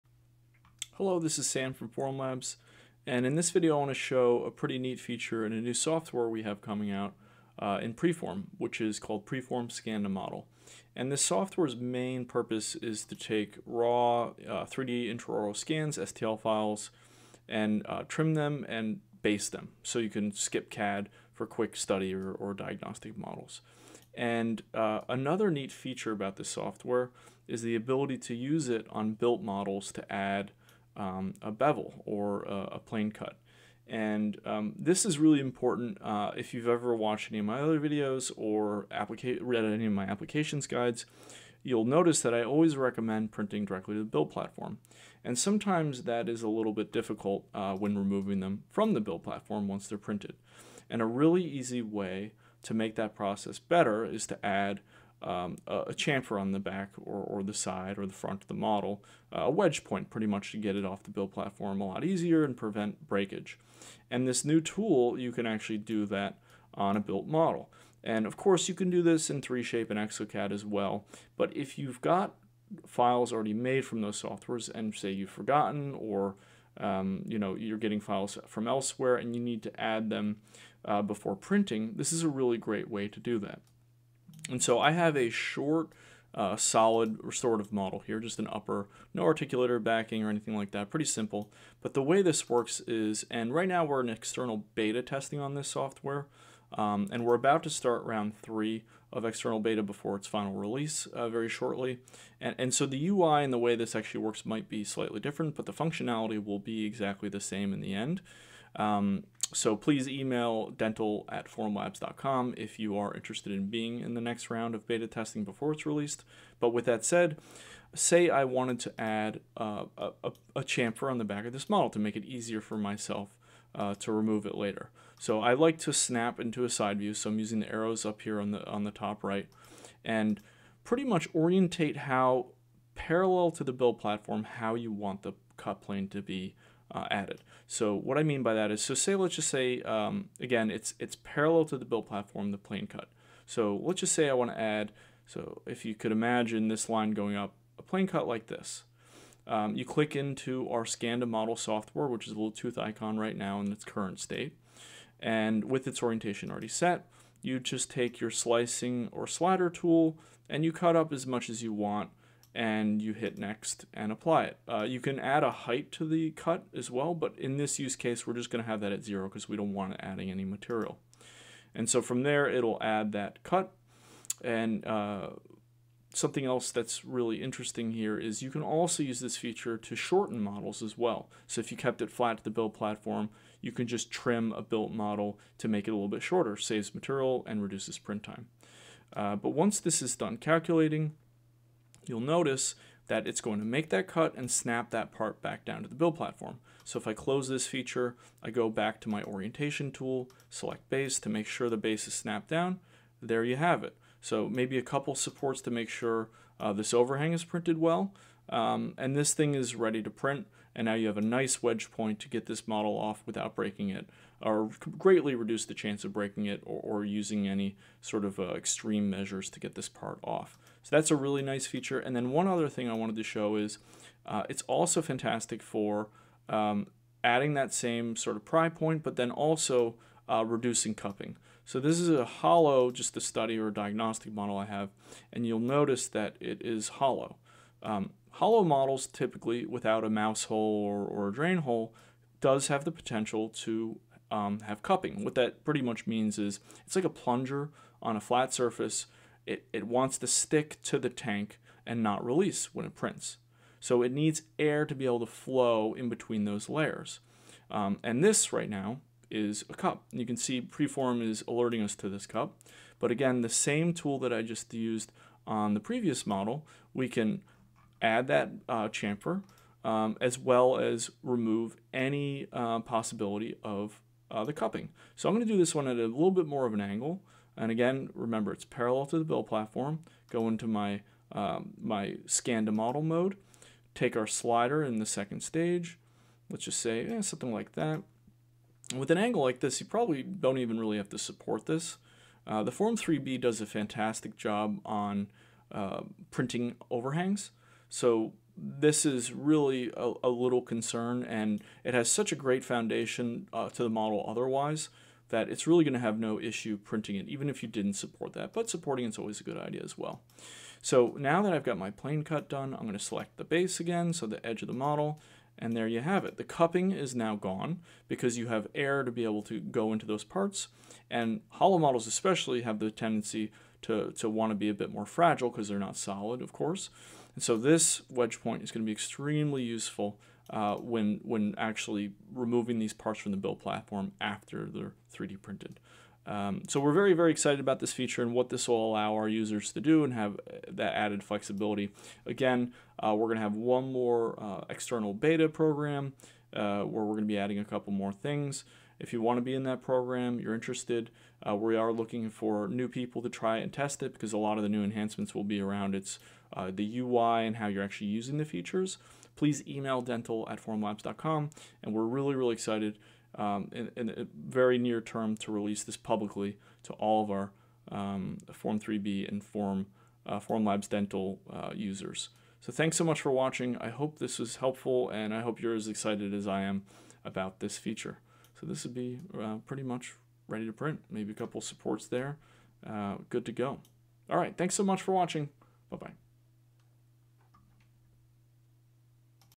Preform Scan to Model - Sneak Peek and Adding Chamfers to Built Models
Interesting in beta testing this new software? Email dental (at) formlabs.com